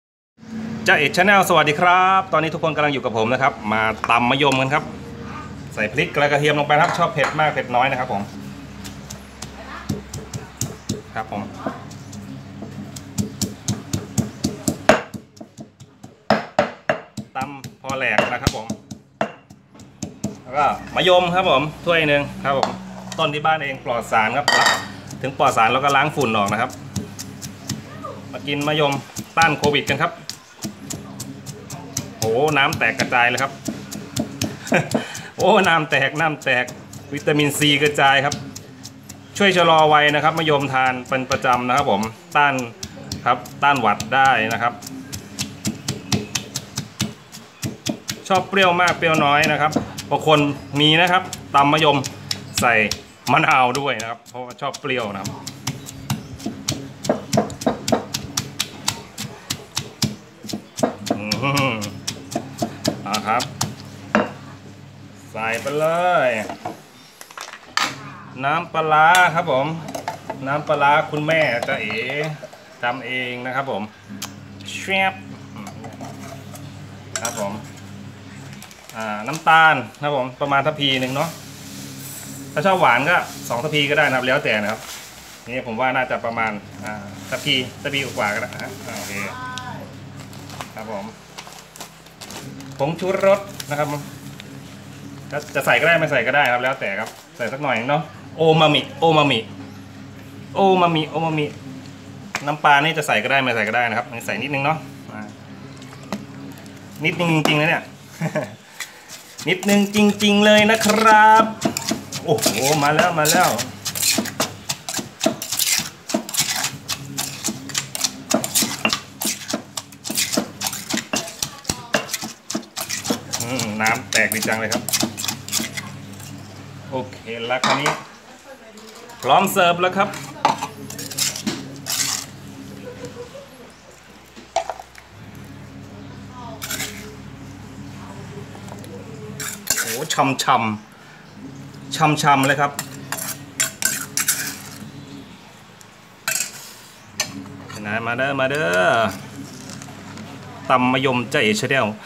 ตำมะยม ยั่วๆ แซ่บๆ
มะยม สรรพคุณ มากมาย ทาน บ่อยๆ เพื่อสุขภาพ แนะนำให้ ทาน พอเหมาะ พอควร นะค้าบ 555++